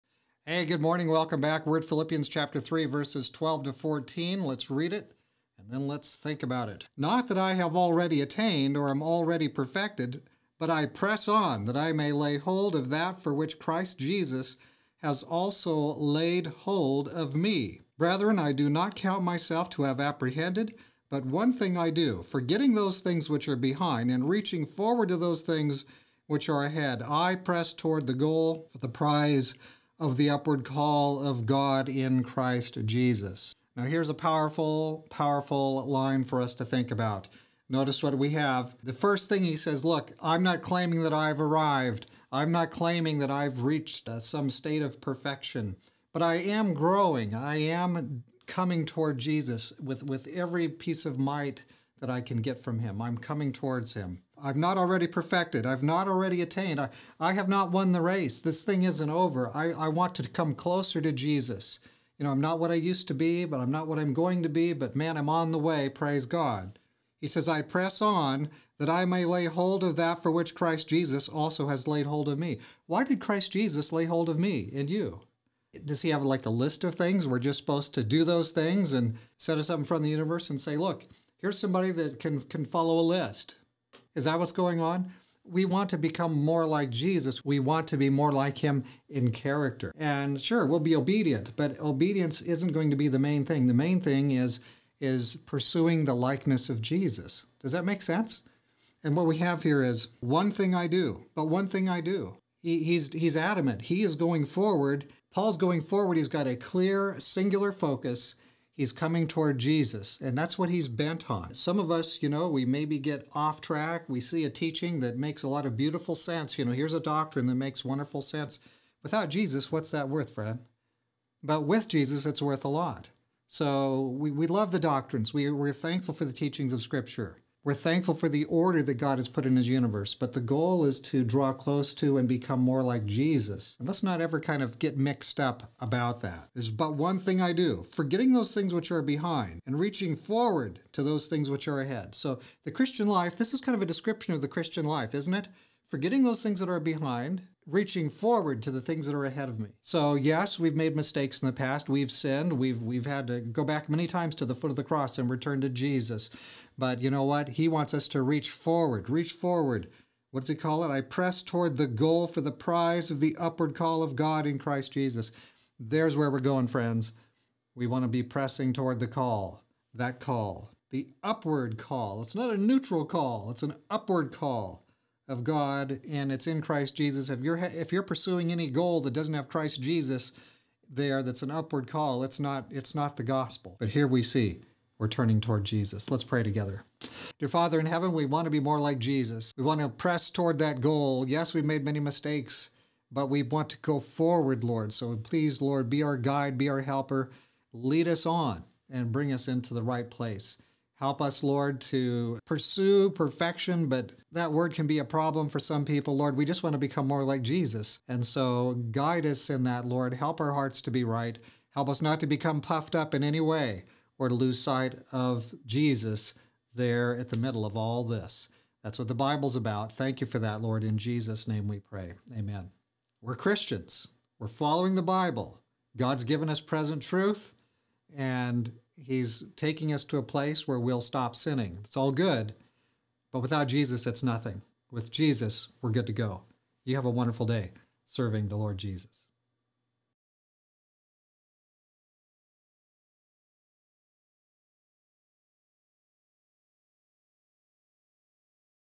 To the Philippians 3:12-14 Pressing On
The Christian life is a forward movement toward Christlikeness.  Where is the stopping place?